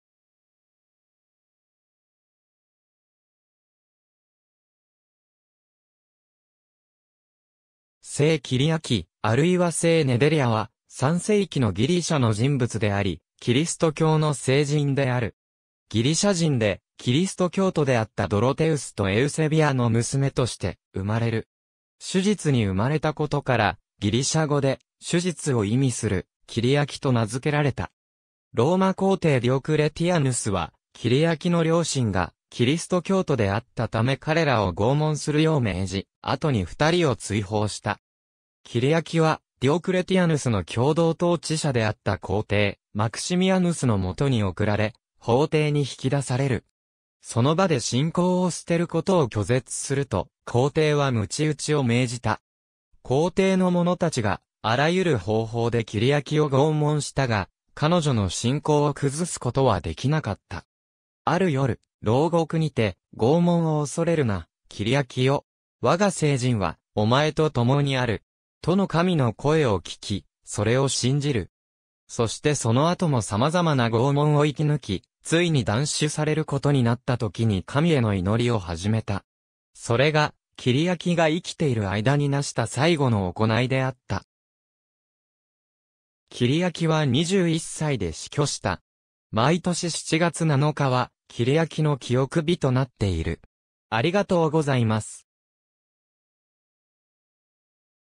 聖キリアキ
聖キリアキ（ギリシャ語:Κυριακή / Kyriakē）、あるいは聖ネデリャ（ブルガリア語:Света Неделя / Sveta Nedelya）は、3世紀のギリシャの人物であり、キリスト教の聖人である。
ギリシャ人でキリスト教徒であったドロテウスとエウセビアの娘として生まれる。
主日（日曜日）に生まれたことから、ギリシャ語で「主日」を意味するキリアキと名づけられた。
ローマ皇帝ディオクレティアヌスはキリアキの両親がキリスト教徒であったため彼らを拷問するよう命じ、後に2人を追放した。
キリアキは、ディオクレティアヌスの共同統治者であった皇帝マクシミアヌスの元に送られ、法廷に引き出される。
その場で信仰を捨てることを拒絶すると、皇帝は鞭打ちを命じた。
皇帝の者たちがあらゆる方法でキリアキを拷問したが、彼女の信仰を崩すことはできなかった。
ある夜、牢獄にて「拷問を恐れるな、キリアキよ。我が聖神（聖霊）はお前と共にある。」との神の声を聞き、それを信じる。
そしてその後もさまざまな拷問を生き抜き、ついに断首されることになったときに神への祈りを始めた。
それがキリアキが生きている間に成した最後の行いであった。
キリアキは21歳で死去した。
毎年7月7日はキリアキの記憶日となっている。